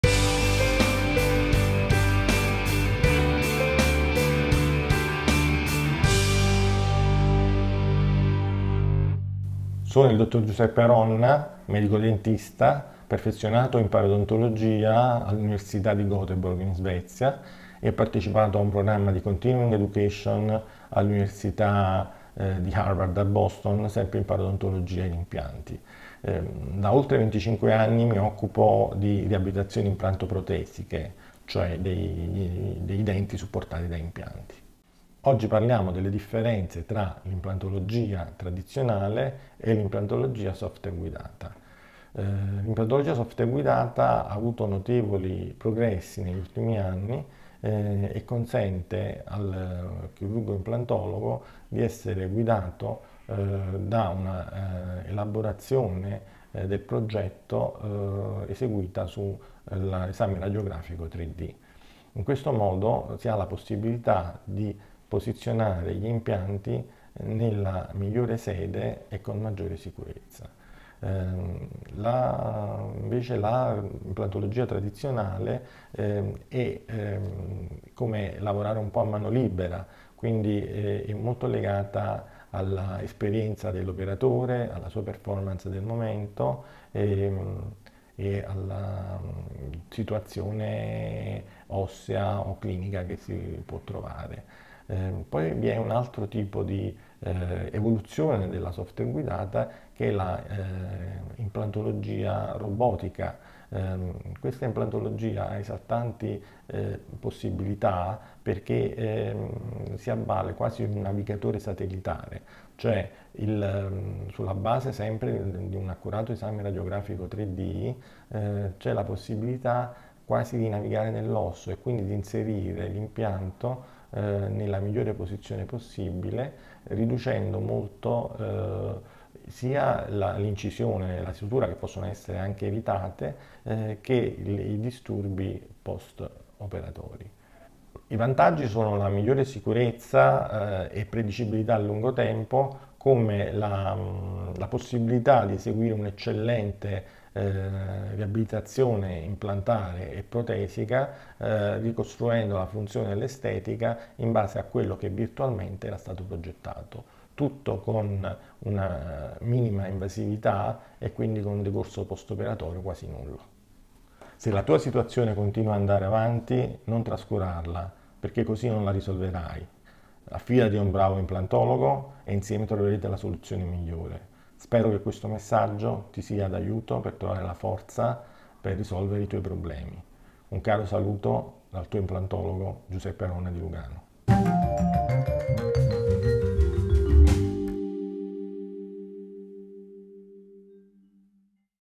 Puntata 4 Implantologia Software guidata vs Implantologia tradizionale - dottor Giuseppe Aronna
Video 4
Implantologia Software guidata vs Implantologia tradizionale

La tecnica Software guidata è una metodica, che negli ultimi tempi ha avuto esaltanti progressi, essa consente al chirurgo implantologo di farsi ‘ guidare ‘dalla elaborazione del programma radiografico per l’inserimento  dell’impianto nella migliore sede possibile sia per l’anatomia dell’ osso mascellare che  per la posizione ideale del dente protesico. Mediante l’uso di mascherine con guide per l’inserimento dell’impianto. Tutto cio’ presenta enormi vantaggi nel posizionamento corretto degli impianti e nella minima invasività della chirurgia che in alcuni casi si può’ eseguire senza scollamento gengivale  con minimo o nessun fastidio post operatorio.
Mentre nella implantologia tradizionale si opera un po’ a mano libera il risultato dipende dall’esperienza dell’operatore e dalla necessità di un più’ ampio campo operatorio
Una ulteriore evoluzione  è l’implantologia robotica computer guidata. Il progresso è dato dalla simulazione sulle immagini radiografiche 3d   del posizionamento dell’impianto e del dente  e tale simulazione diviene realtà nel mascellare  grazie ad una sorta di navigatore satellitare che guida la dinamica dell’inserimento dell’impianto :sul monitor si vede l’avanzamento  dell’impianto nell’alloggiamento osseo. I vantaggi sono la migliore sicurezza e l’eccellente riabilitazione implantoprotesica sotto il profilo funzionale ed estetico, la minima invasività :ill paziente al termine dell’intervento puo’recuperare il piacere di sorridere subito senza alcun fastidio.